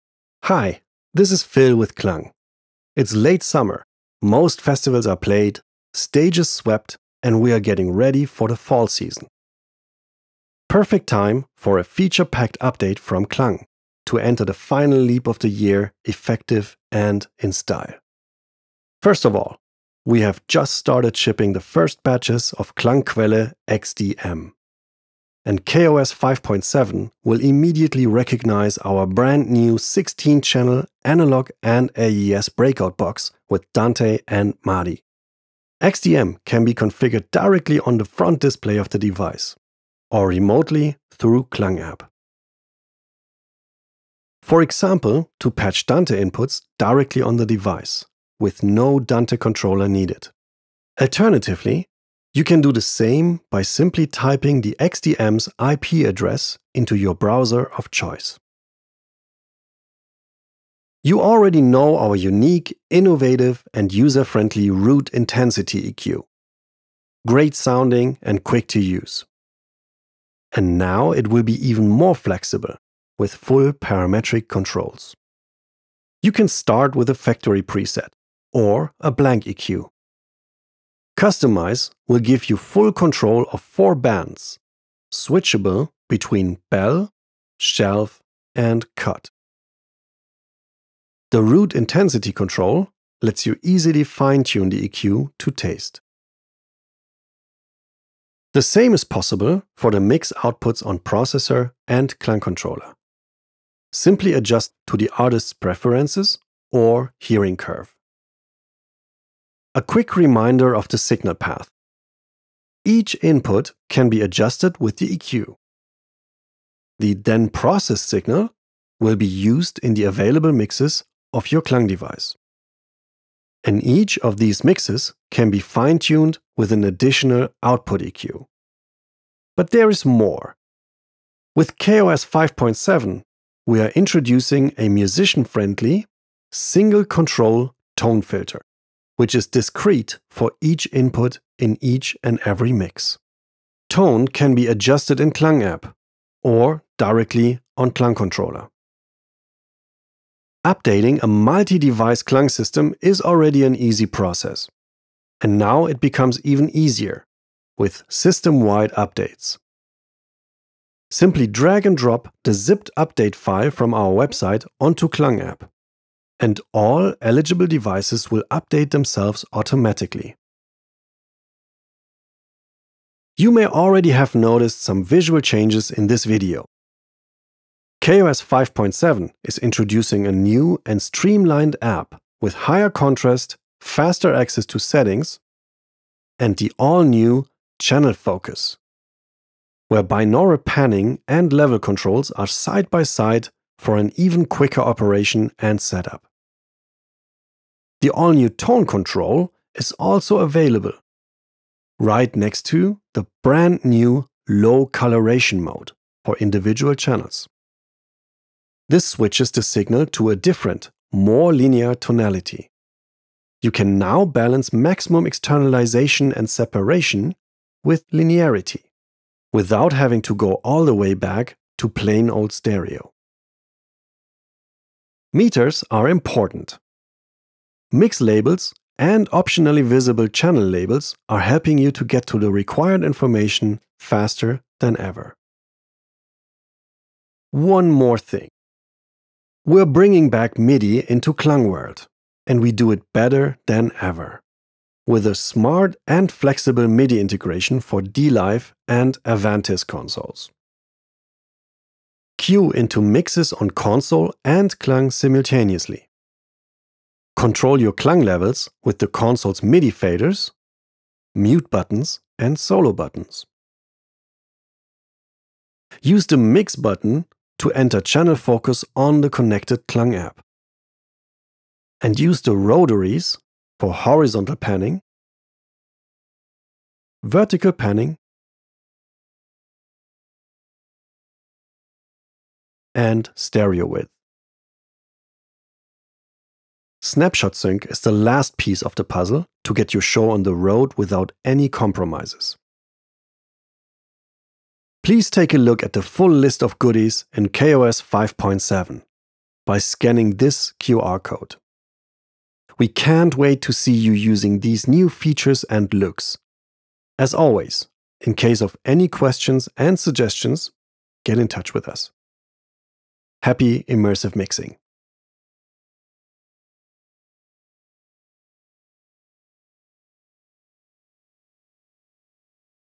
What's new in KOS 5.7?
Discover what’s new in KOS 5.7, the latest software update for KLANG's full product range.
This release introduces powerful enhancements that make your workflow faster, your mixes more personal, and your control more flexible.

Key highlights of KOS 5.7:
- Fully customizable parametric EQs for input channels and mix outputs
- Personal tone filters per input channel per mix for a truly tailored sound
- New MIDI integration with Allen & Heath dLive and Avantis consoles
- Advanced navigation tools, including Channel Focus in STAGE view

Whether you’re a monitor engineer or a musician, KOS 5.7 gives you more precision, clarity, and control than ever before.

Introduction: 0:00
KLANG:quelle XDM: 0:21
Custom Parametric EQ: 1:00
Personal Tone Filter: 1:50
System Wide Updates: 2:30
UX Updates: 2:52
Channel Focus: 3:08
Low Coloration Mode: 3:24
Labels for Meters: 3:45
Allen & Heath MIDI Integration: 3:55